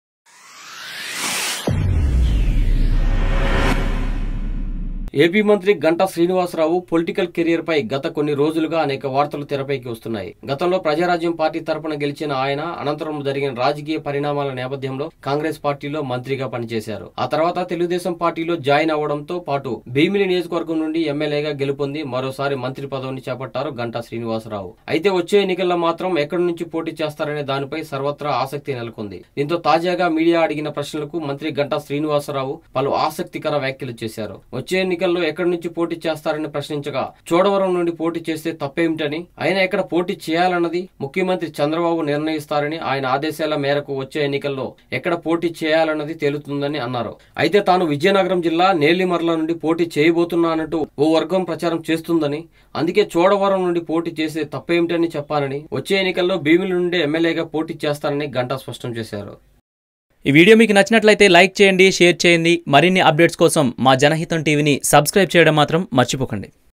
Ganta sinivasara rao give clarity on contesting 2019 elections || janahitam tv
Ganta sinivasara rao give clarity on contesting 2019 elections